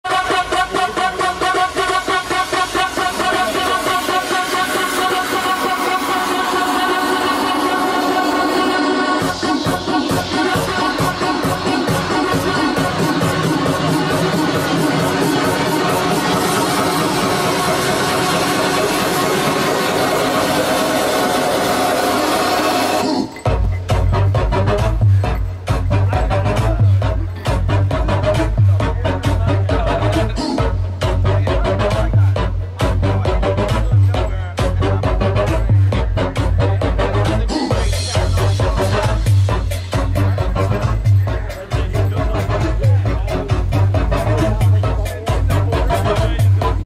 saphi dancing around
another video from the motorfurs event at eurofurence 23.. saphi just dancing around